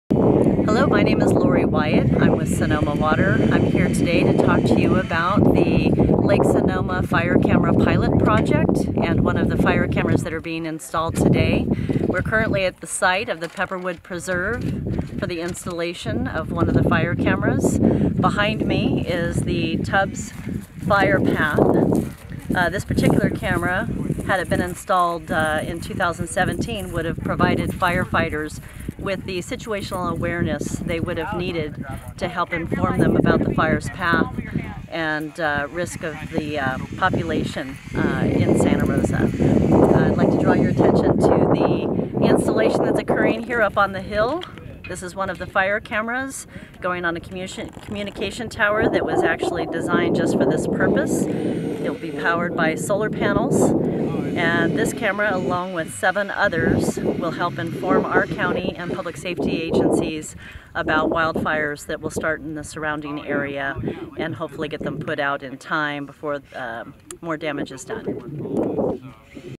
Fire Camera Project
Lori Wyatt, of Sonoma Water, talks about the Fire Camera Project.